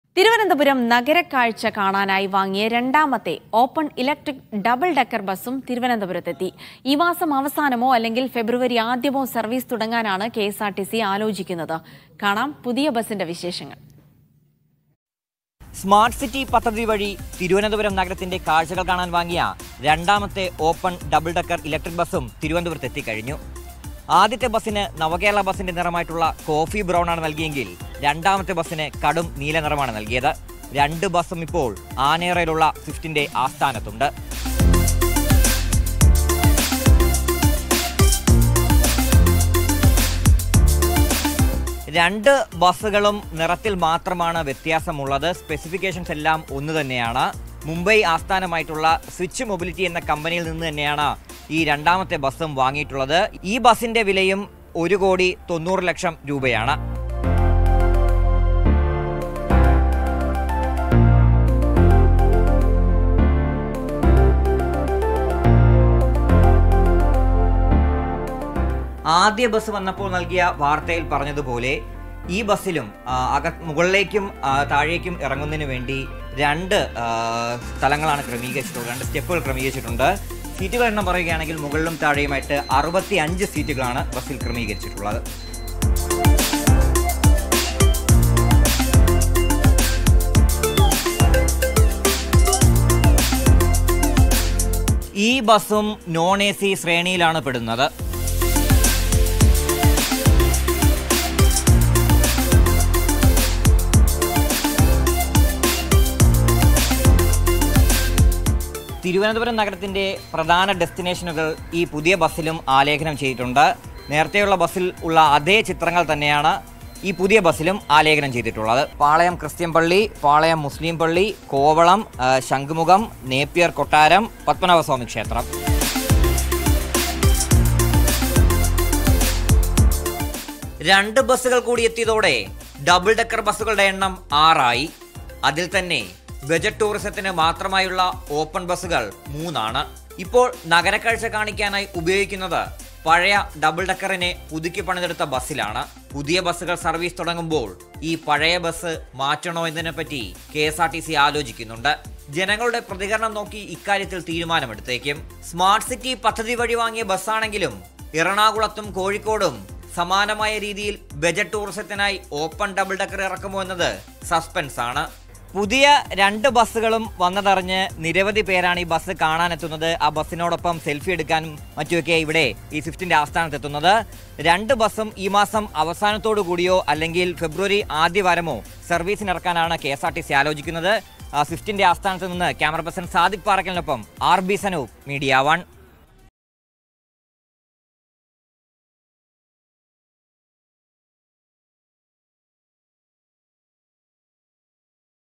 KSRTC ഇലക്ട്രിക് ഡബിള്‍ ഡക്കര്‍; നഗരക്കാഴ്ചയ്ക്കായി രണ്ടാമത്തെ ബസും തിരുവനന്തപുരത്ത്
KSRTC ഇലക്ട്രിക് ഡബിള്‍ ഡക്കര്‍; നഗരക്കാഴ്ചയ്ക്കായി രണ്ടാമത്തെ ബസും തിരുവനന്തപുരത്ത് | KSRTC | Electric Double Decker bus |